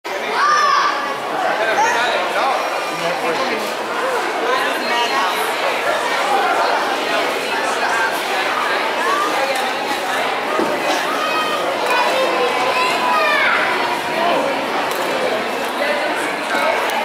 FAO Schwarz Fifth Avenue, NY
A view of the inside store for 2011 holidays.